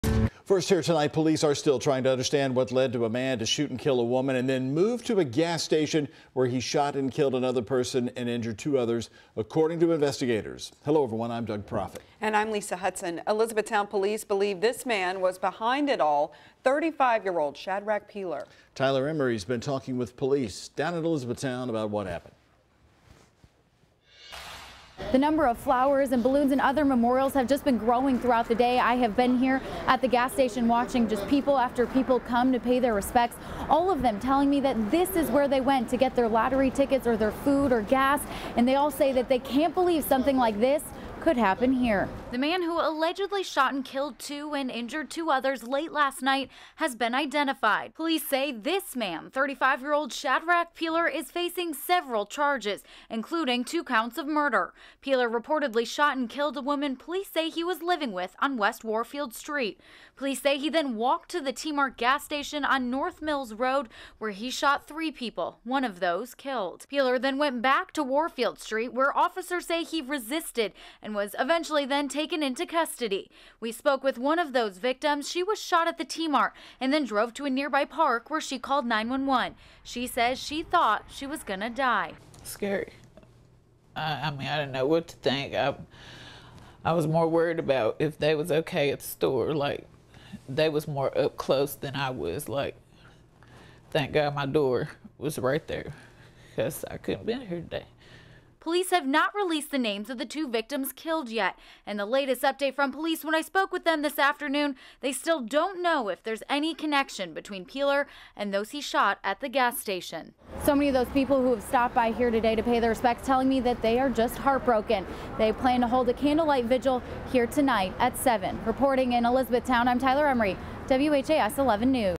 2 dead, 2 hurt in Elizabethtown shootings
Police are still trying to understand what led a man to shoot and kill a woman and then move to a gas station where he shot and killed another person and injured two others.